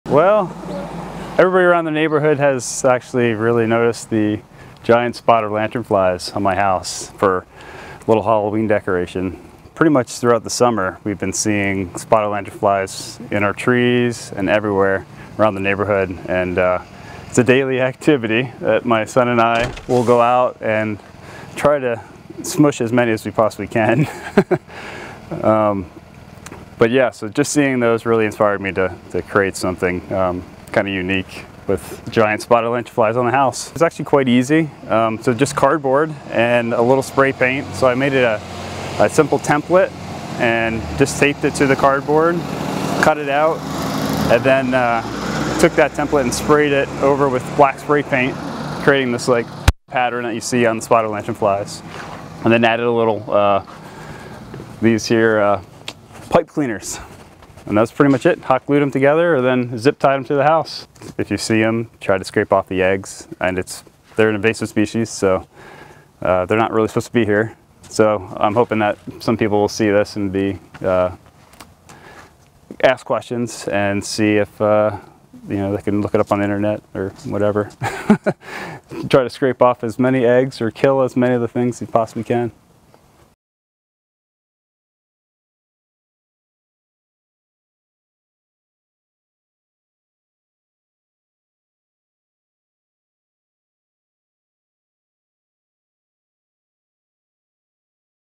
Giant Spotted Lanternflies cover this central Pa. home as Halloween decoration
John Lamb, of 168 Walden Way in Mechanicsburg, took the opportunity this Halloween to raise awareness about an invasive species in the area, decorating his home in spotted lanternflies. Watch to get a first look!